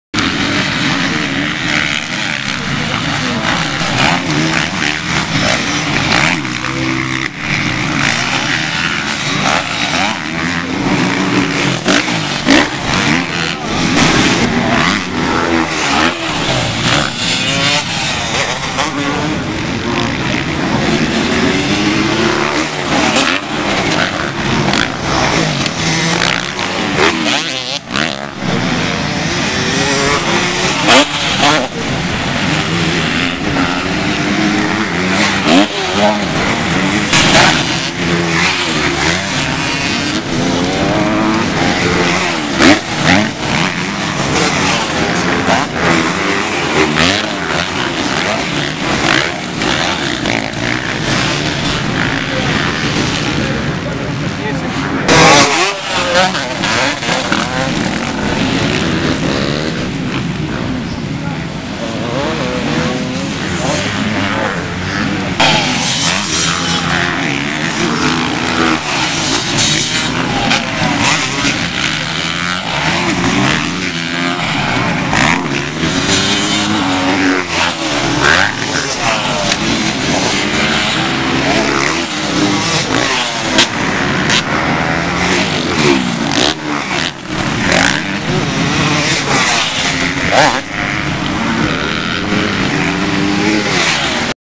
motocross tuzie 25 9 2016 trophee 4t 3e manche
Tuzie Charente 25 septembre 2016 3e manche finale Trophée 4T régional UFOLEP.